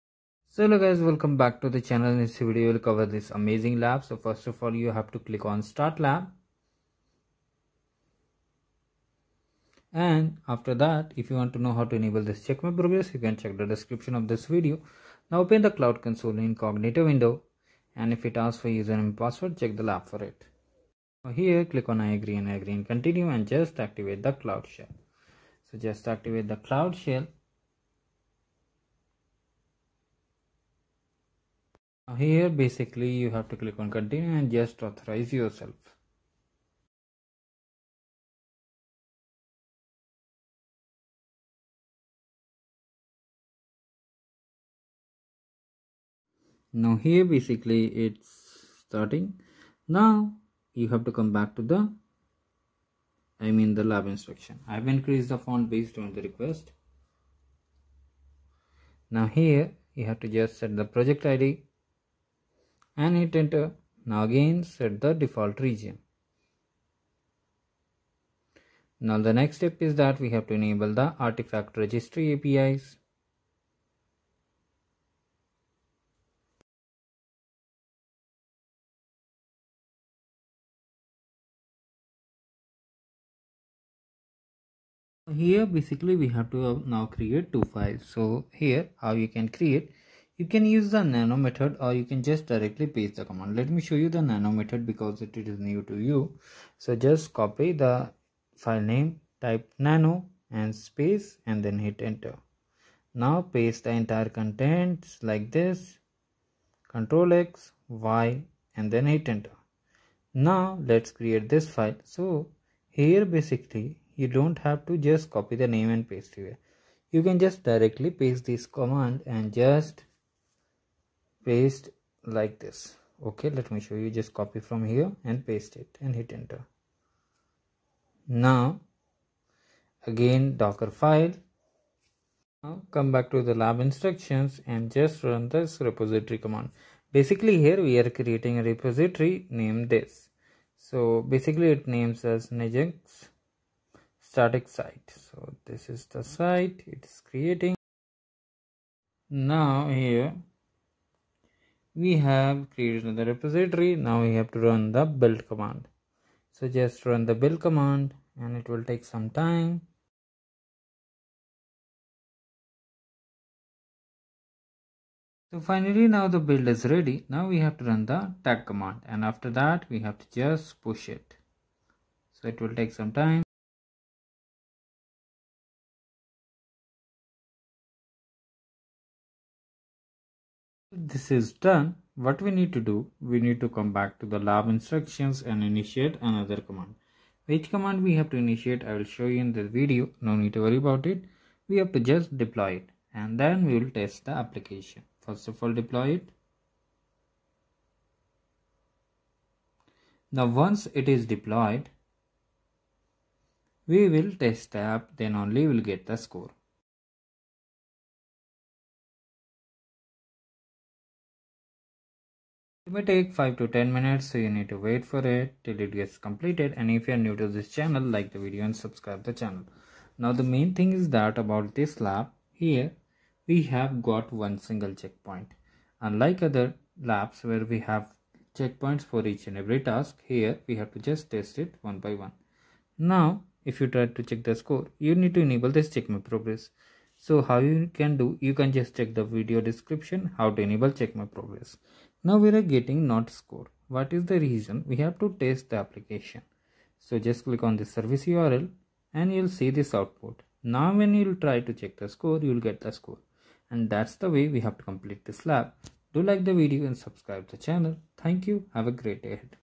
Deploy a Static Site with Nginx on Google Cloud Run using Artifact Registry |Level 3
Deploy a Static Site with Nginx on Google Cloud Run using Artifact Registry |Level 3 | qwiklabs gem-cloud-run-nginx-website

🔗 Join the Journey:
📚 Learning Resources

🧠  Lab URL -
📛 Skill badge

🔍 Common Queries
qwiklabs
arcade facilitator